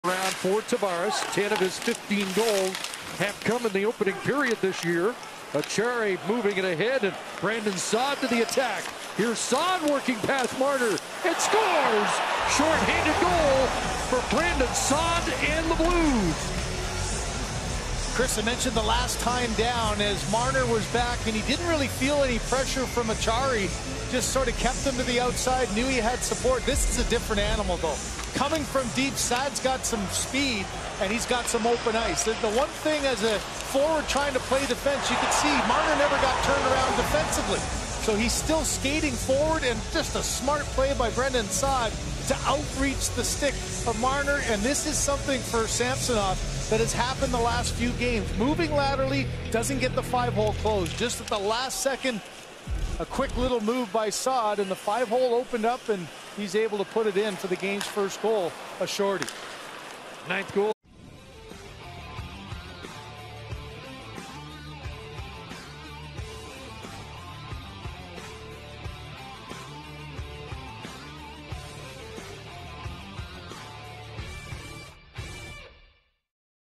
Blues' Brandon Saad Outduels Mitch Marner To Score Shorthanded Marker vs. Maple Leafs
Watch as St. Louis Blues forward Brandon Saad speeds through the zone for a one-on-one situation with Mitch Marner, and scores a shorthanded goal going five hole against Ilya Samsonov.